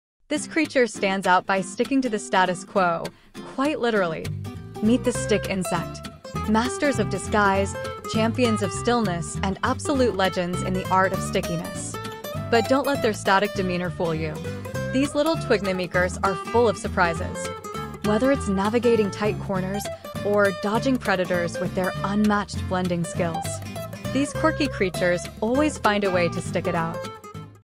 🍃 the secret life of stick insects: nature's stealthy ninjas 🥷
Welcome to the mesmerizing world of stick insects! In this fascinating video, we delve deep into the lives of these remarkable creatures, showcasing their unparalleled skills in camouflage and survival. Watch as we uncover the secrets behind their incredible ability to mimic twigs and leaves, blending seamlessly into their surroundings.

Join us on a journey through dense forests and lush greenery as we observe stick insects in their natural habitats. From the tiniest nymphs to the largest adults, every member of this unique insect family has its own story to tell.

Through stunning footage and expert insights, you'll gain a newfound appreciation for these masters of disguise. Learn how stick insects evade predators with their stealthy tactics and adapt to a variety of environments.

But it's not all serious business! Prepare to be entertained as we showcase the lighter side of stick insect life, from their quirky movements to their hilarious attempts at navigating obstacles.

Whether you're a nature enthusiast or simply curious about the wonders of the insect world, this video promises to captivate and educate. Join us as we unravel the mysteries of stick insects and celebrate the beauty of nature's most intriguing camouflage artists! 🌿🎥